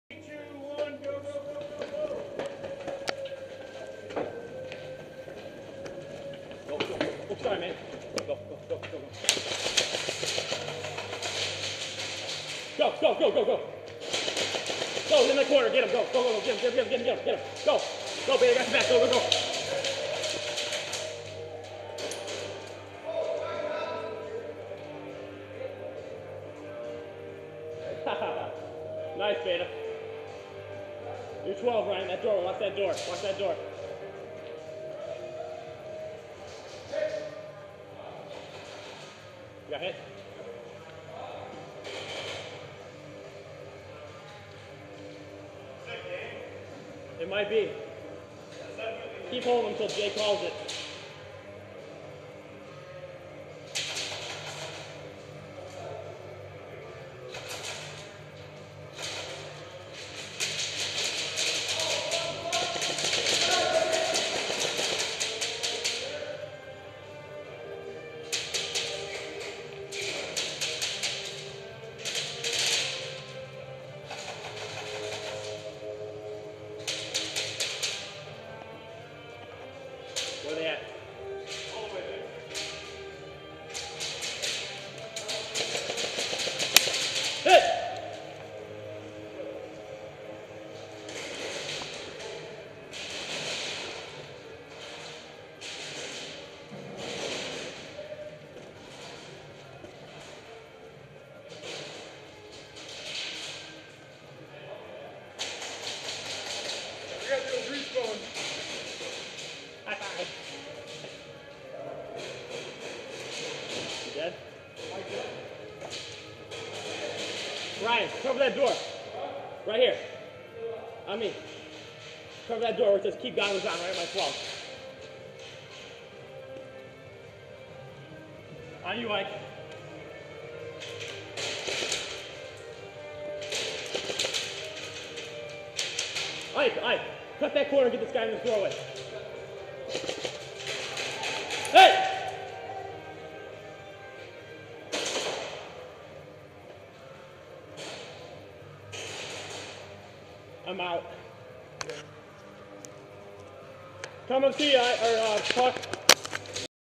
ODA 561 Force on Force #1 7/23/11 Airsoft CQB Michigan Airsoft
Force on Force game at Phoenix Tactical's indoor field.

1 re-spawn (2 lives) per player, play until one team is dead.